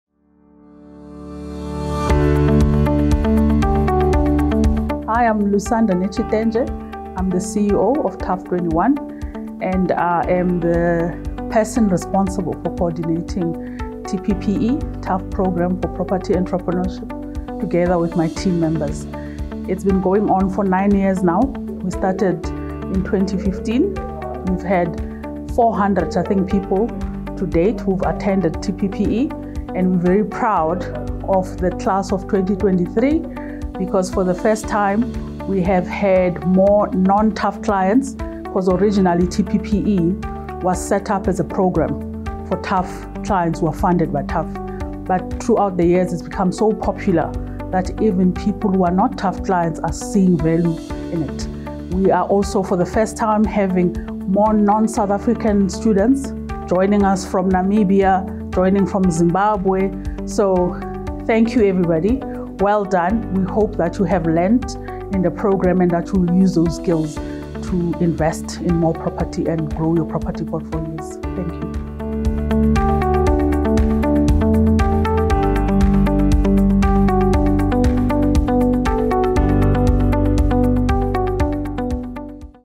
Lusanda Netshitenzhe, CEO of TUHF21: Highlights from the TPPE Graduation Ceremony 2023
Lusanda Netshitenzhe, CEO of TUHF21, shares some of the impressive achievements that the TUHF Programme for Property Entrepreneurship (TPPE) celebrated as part of the graduation ceremony for the Class of 2023. Check out the full interview.